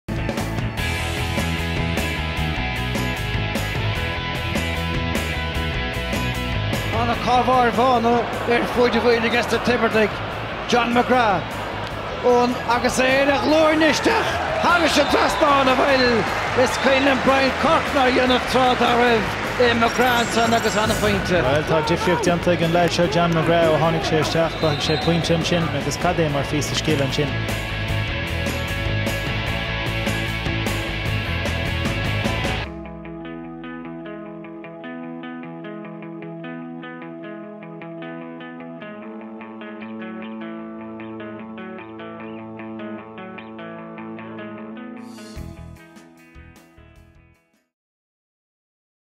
Tipperary vs Cork (Munster Semi-Final): Score of the Game from John McGrath of Tipp
No it's not Brian Corcoran it's John McGrath from Tipp scoring a wonder point from his knees in the 2013 Bord Gáis Energy GAA U21 Munster Semi-Final between Tipperary and Cork in Semple Stadium.